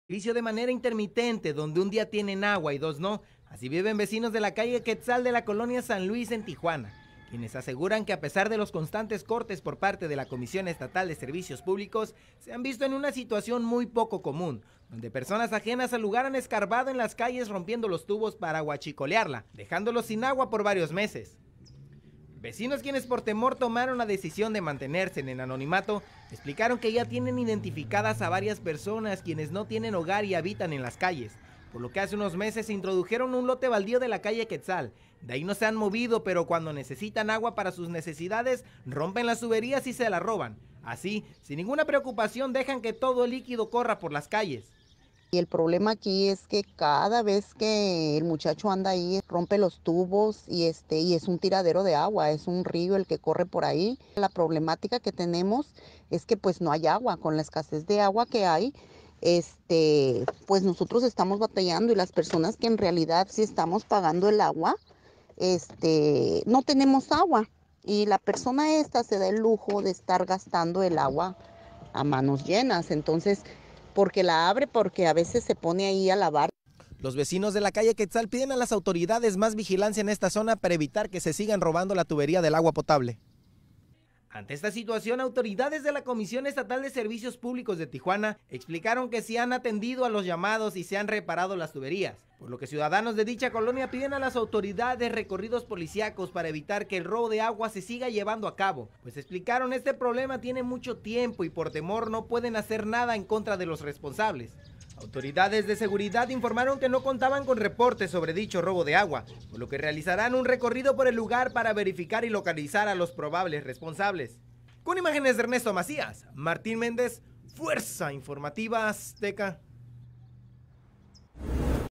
Ciudadanos denuncian robo de agua en su colonia.
Personas ajenas al lugar, escarban y rompen los tubos para robarse el vital líquido.